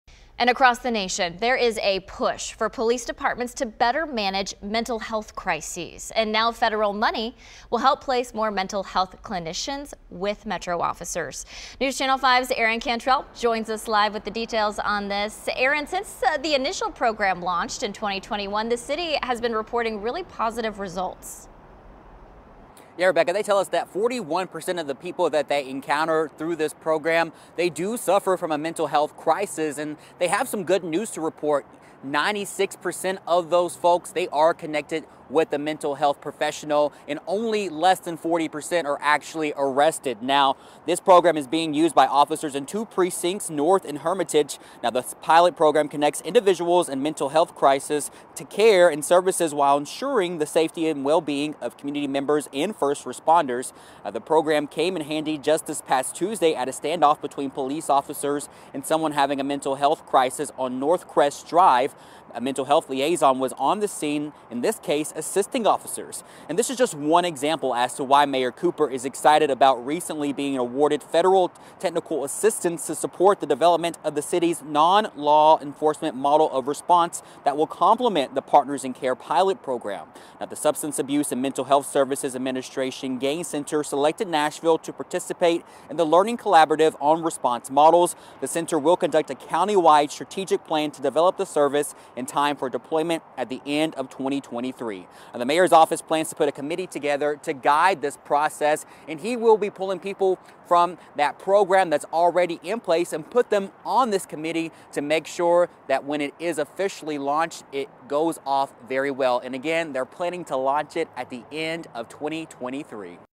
Nashville receives federal aid to support co-response program for MNPD mental health calls
There's been a push for police departments around the country to be better equipped to handle mental health calls.